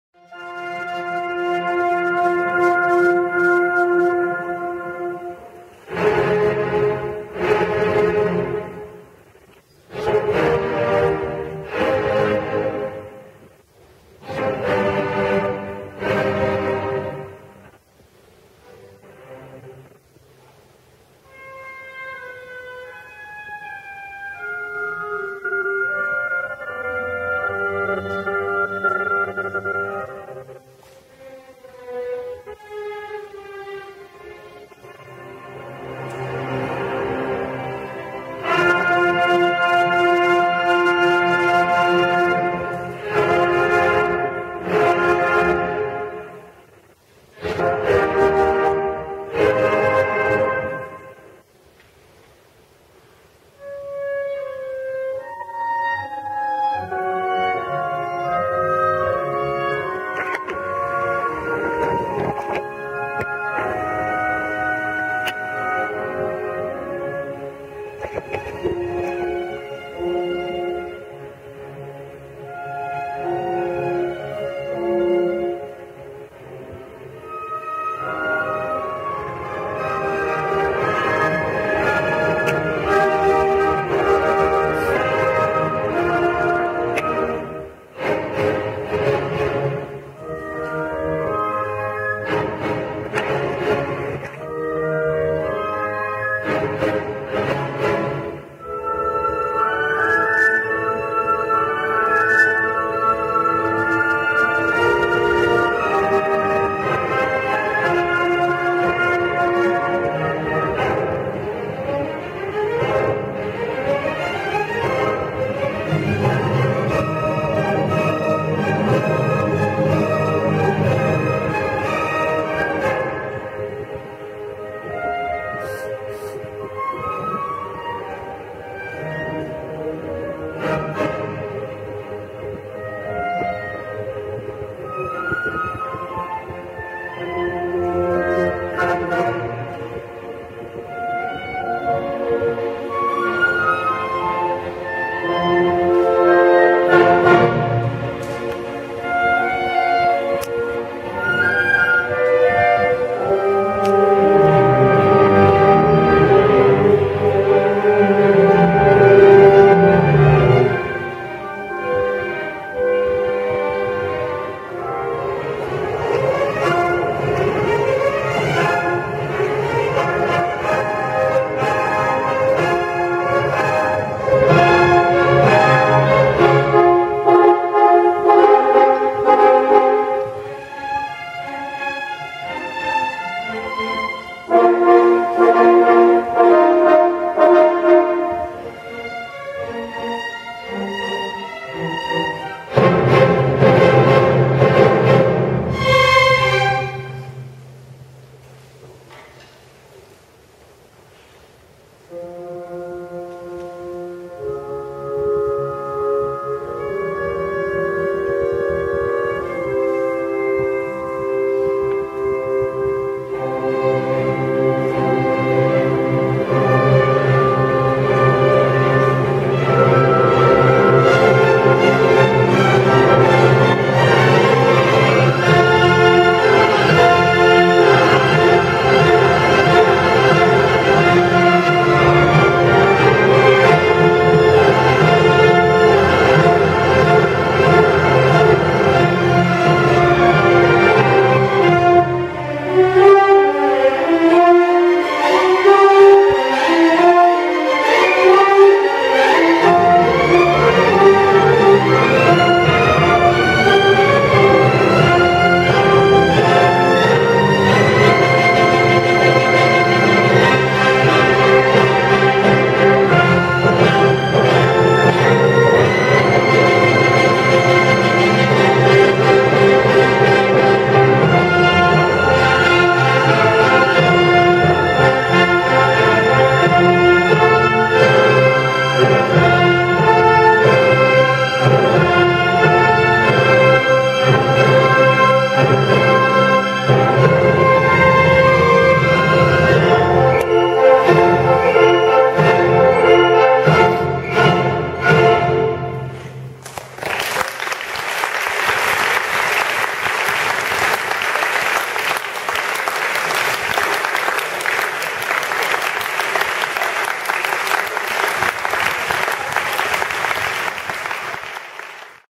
Sonata Encore
SNYO: Short Video Clip of Encore Piece -  ... 18 Mar 2012. (no full video)  - 1st Performance with SNYO, on his 1st Viola.